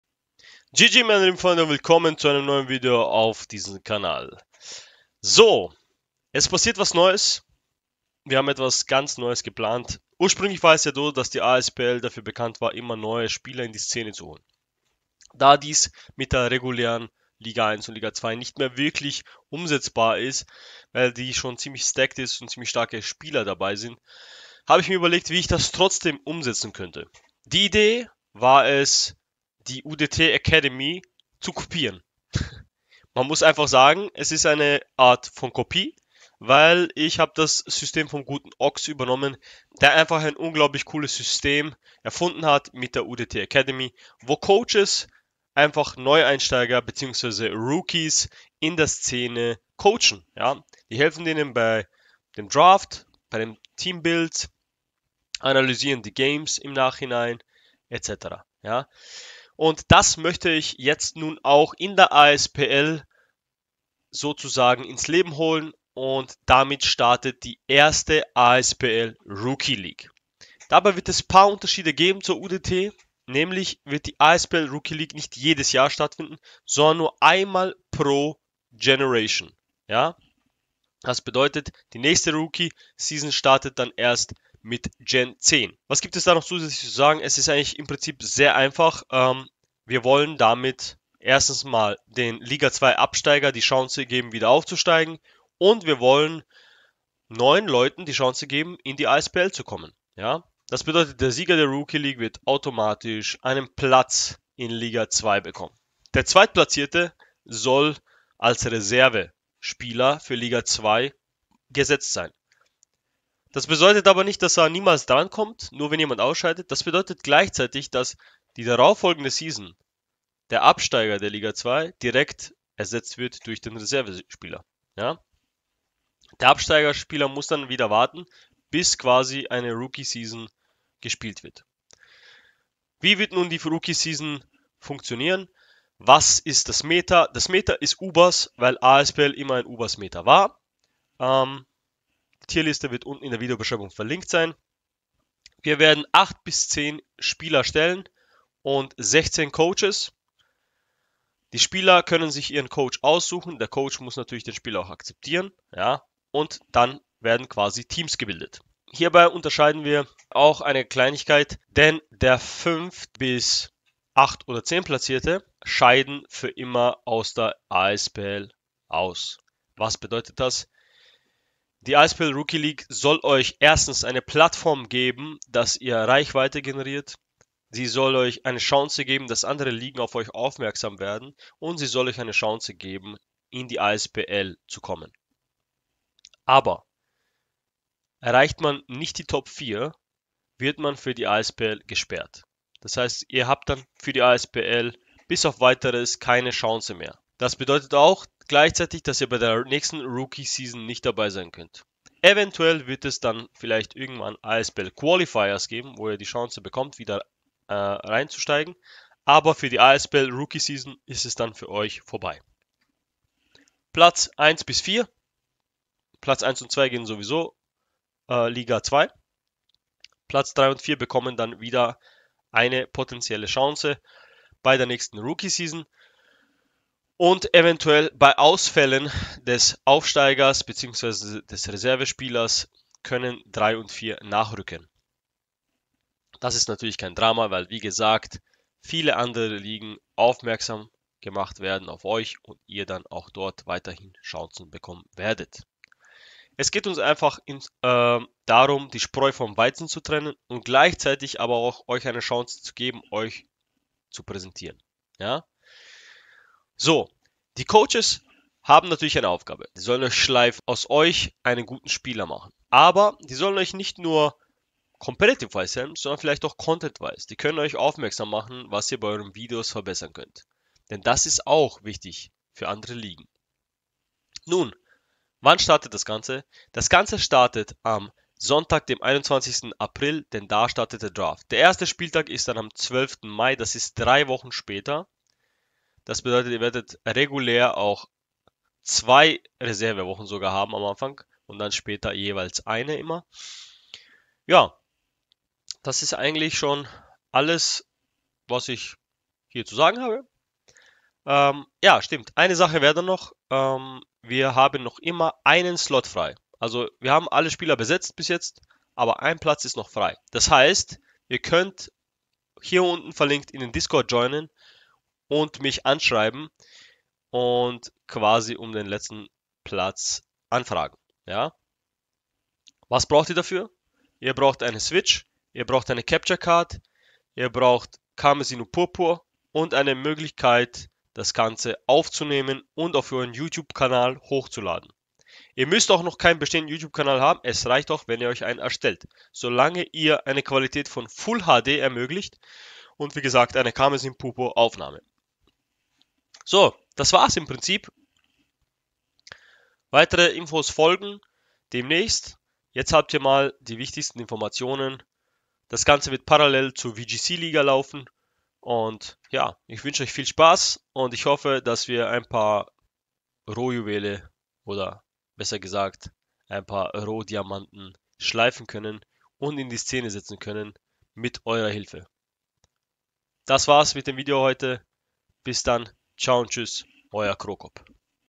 ASPL Rookie League - Ankündigung
GG meine lieben Freunde wir starten rein mit einer weiteren neuen Liga diesmal für Rookies.